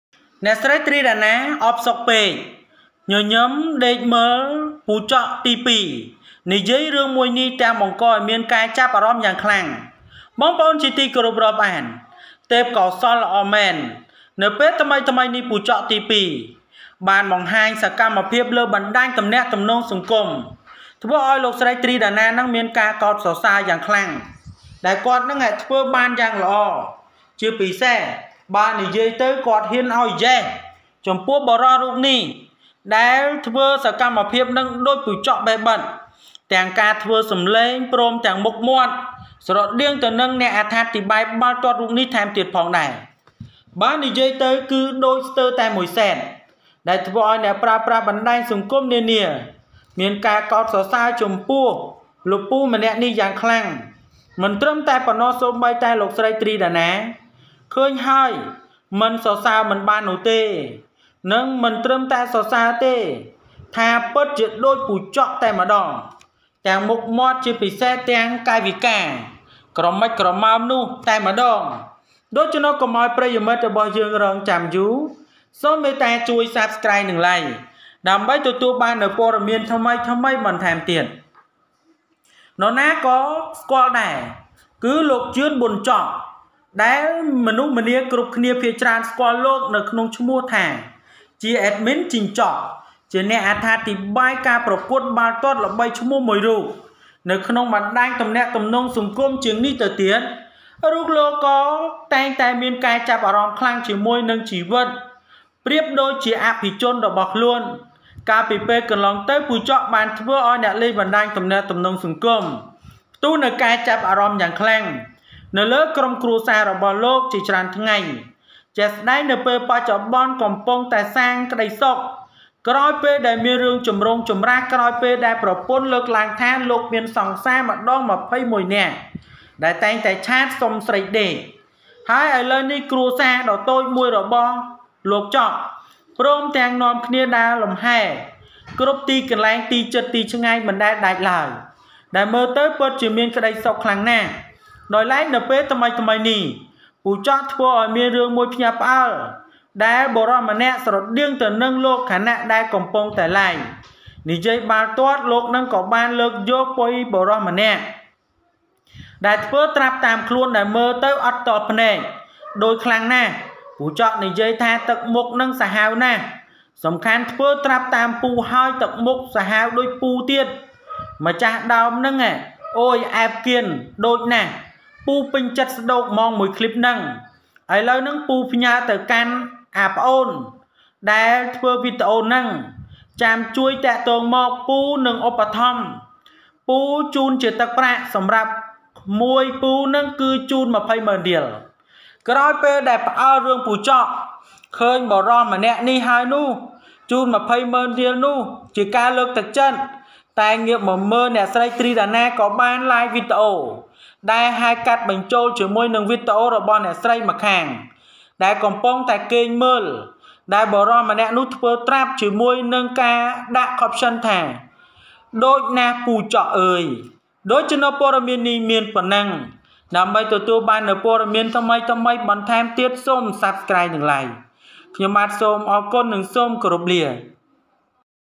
ទ្រី ដាណា អផ្សុកពេក ញញឹម ដេកមើល ពូចក់ទី២ និយាយរឿង
ទ្រី ដាណា អផ្សុកពេក ញញឹម ដេកមើល ពូចក់ទី២ និយាយរឿង Try Dana is too bored, smiles, sleeps, watches her second uncle tell a story.